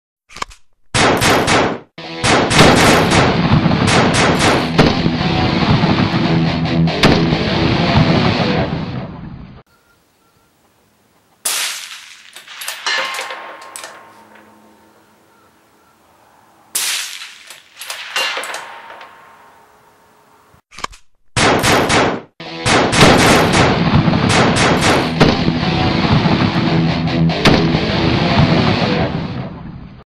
Ring The Bell
Just goofing off at 375 yards.    The Rifle used is a custom built 260 Rem.  put together by Mark @ Short Action Customs.   Scope is a S&B  PMII  5-25 ,  ammo 140 Bergers  custom handloads.